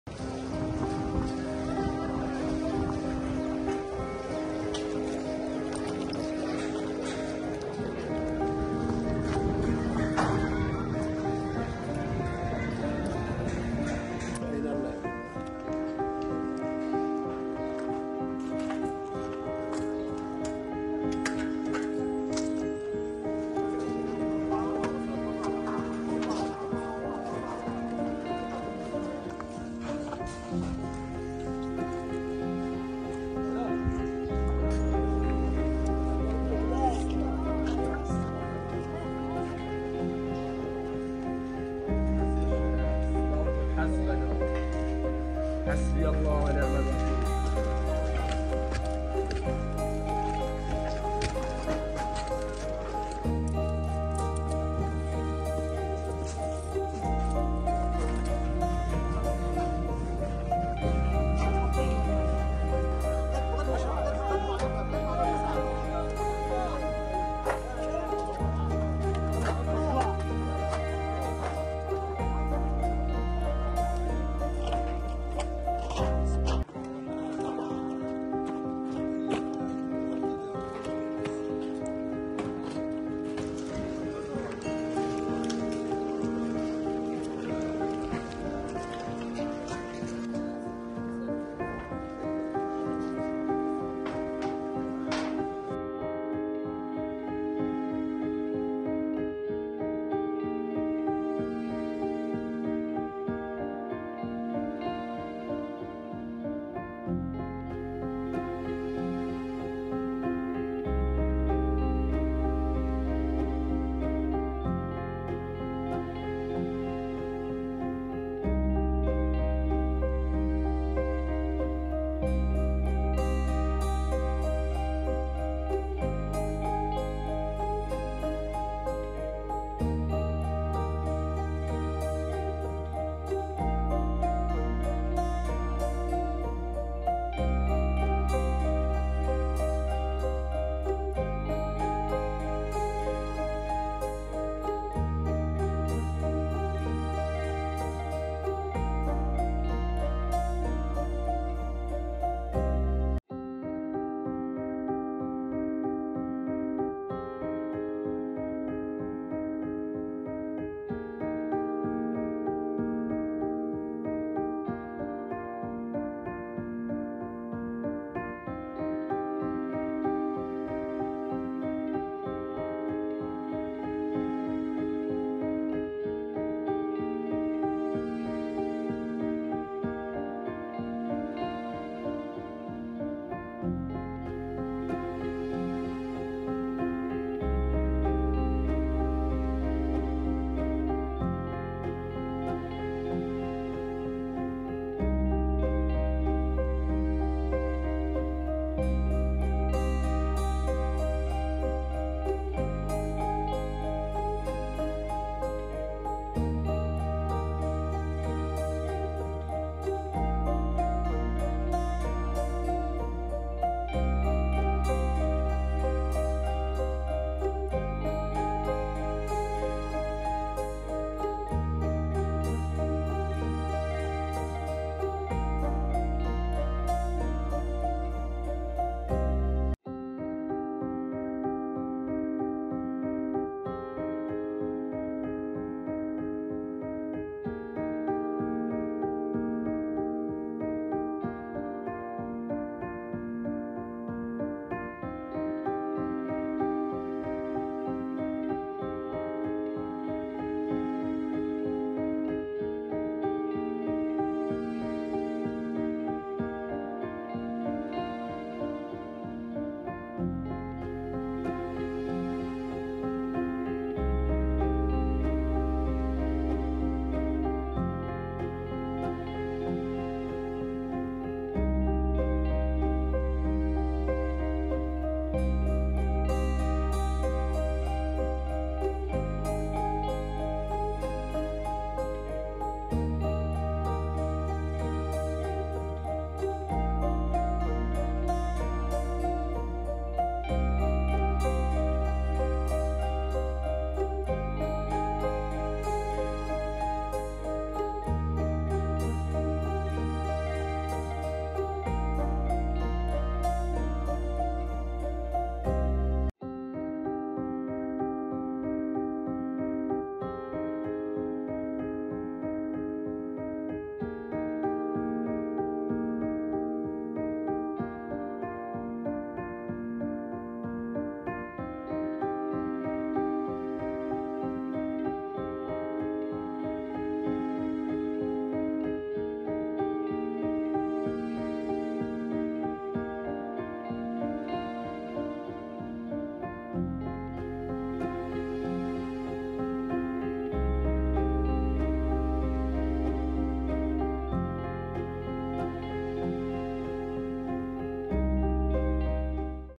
Drone Footage Shows Gaza Devastation Amid Israeli Bombardment : ড্ৰোণত বন্দী ভয়াৱহ দৃশ্য | N18V
Drone Footage Shows Gaza Devastation Amid Israeli Bombardment | যুদ্ধ বিধ্বস্ত গাজা, ড্ৰোণ কেমেৰাত বন্দী ভয়াৱহ দৃশ্য